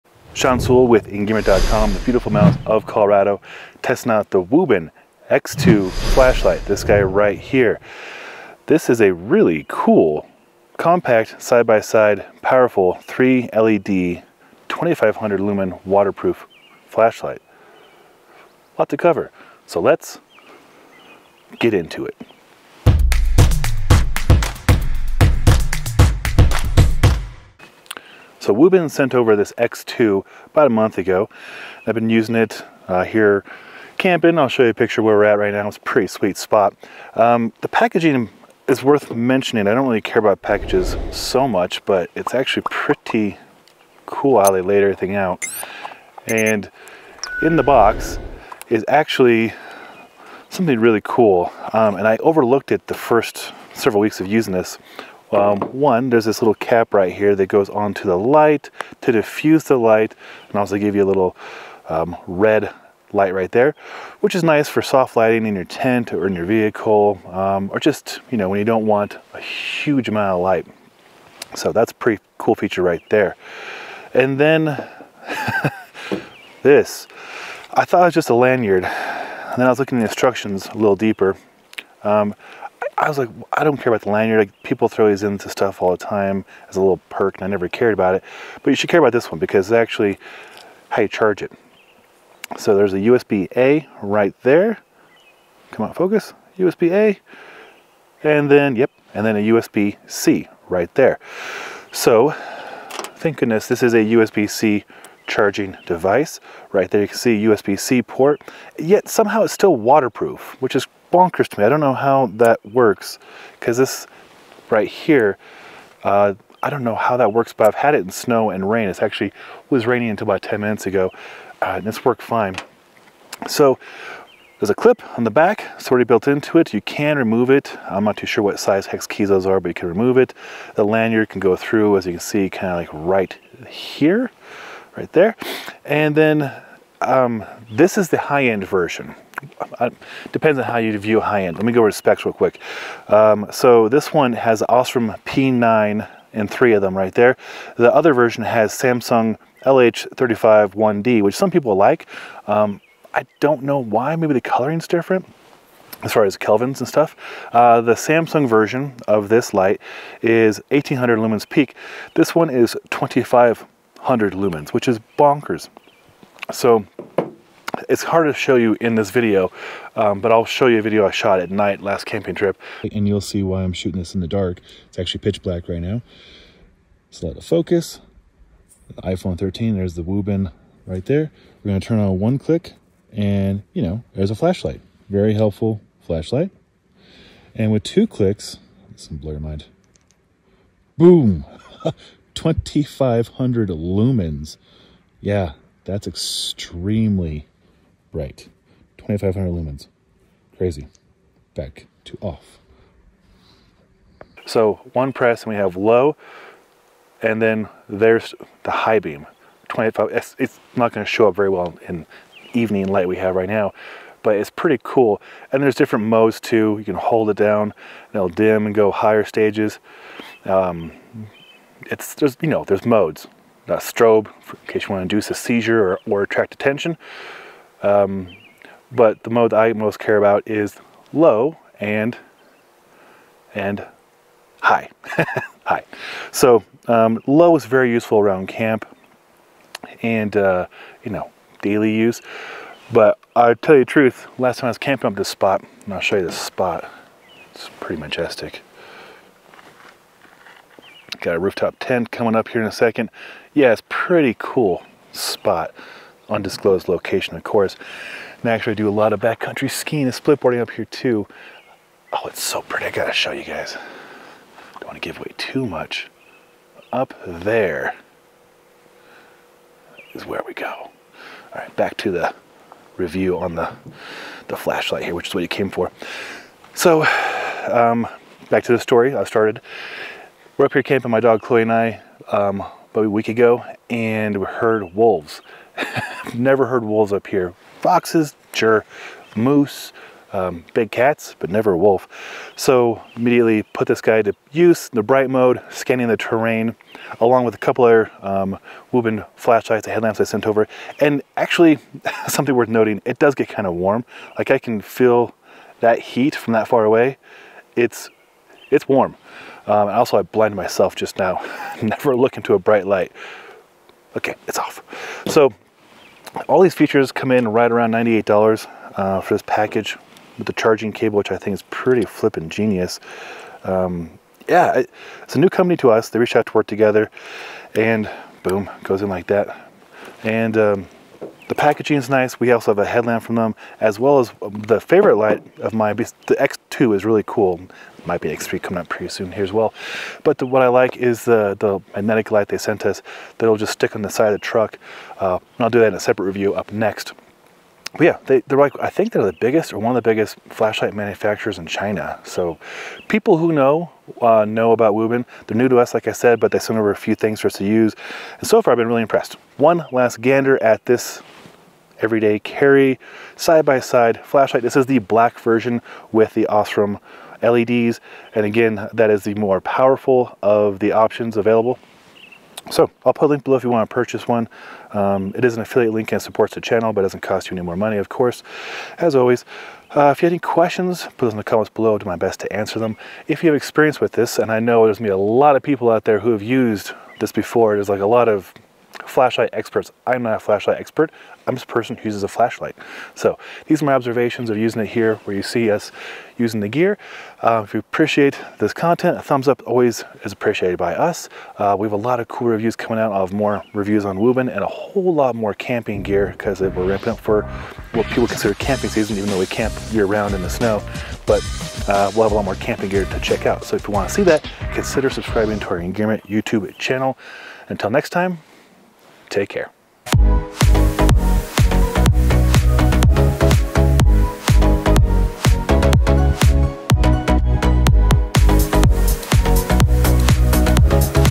Wuben X2 Flashlight Review - 2500 Lumen EDC Flashlight with Unique Design
The Wuben X2 Flashlight is a unique flashlight with 3 high power bulbs. Capable of 2500 lumens at its highest setting.  It is also USB C rechargeable and the included lanyard is also the charging cable.